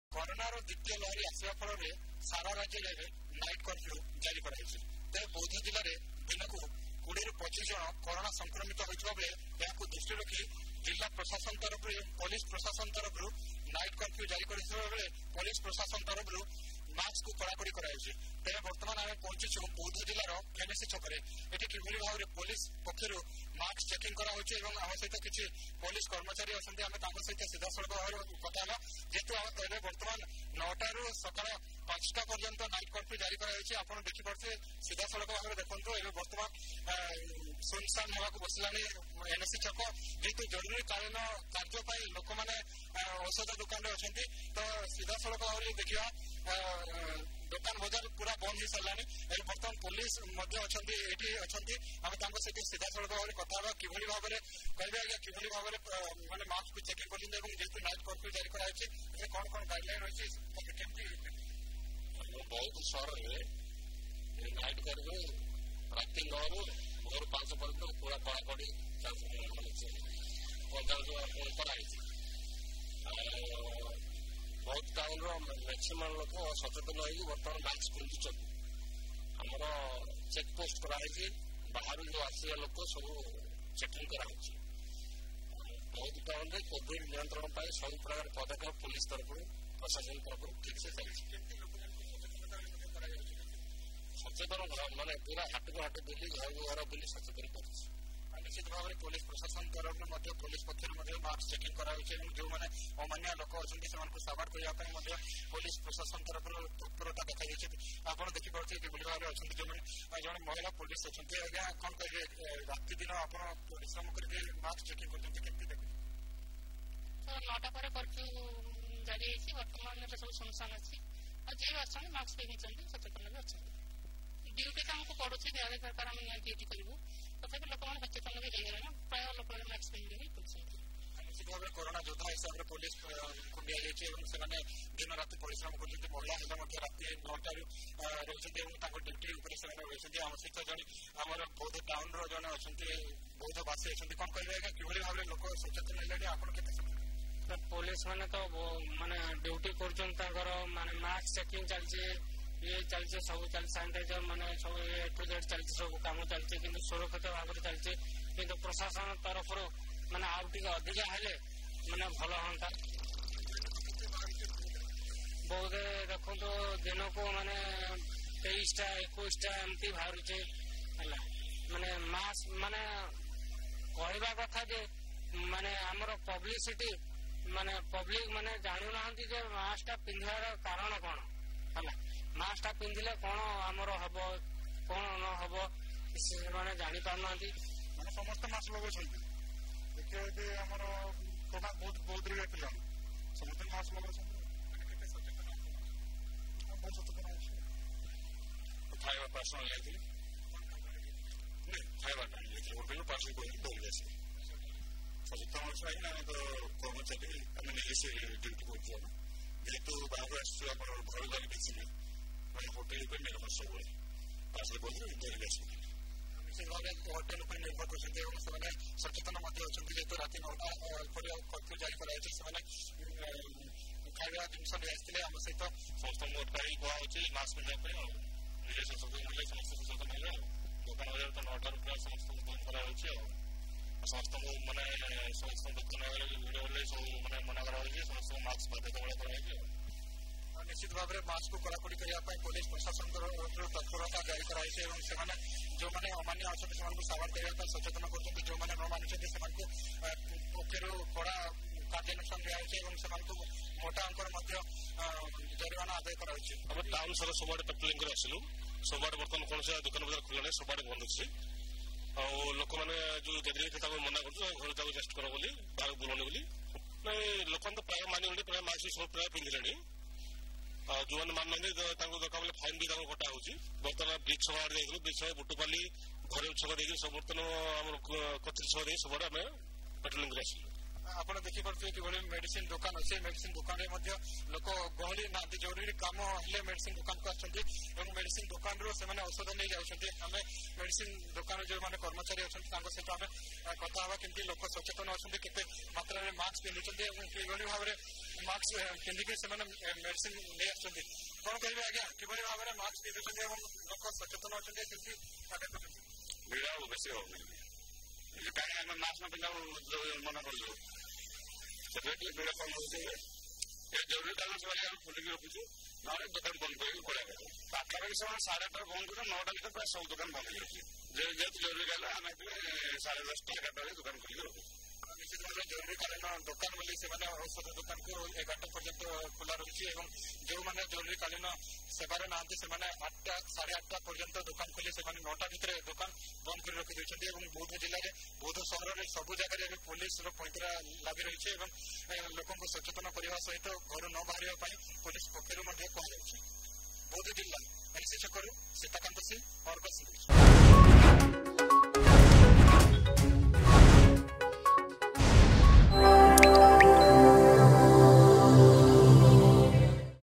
ବୌଦ୍ଧ ଜିଲ୍ଲାରେ ନାଇଟ କର୍ଫ୍ୟୁ ବଳବତର ରହିଛି
ସାରା ରାଜ୍ୟରେ କୋରୋନା କାୟା ବିସ୍ତାର ହେଉଥିବା ବେଳେ...ବୌଦ୍ଧ ଜିଲ୍ଲାରେ ନାଇଟ କର୍ଫ୍ୟୁ ବଳବତର ରହିଛି ପୋଲିସ ପକ୍ଷରୁ ପେଟ୍ରୋଲିଙ୍ଗ କଡା କଡୀ କରାଯାଇଛି ଏବଂ ସମସ୍ତ ଛକ ଗଳିରେ ମାସ୍କ ଚେକିଂ କରୁଛି ପୋଲିସ...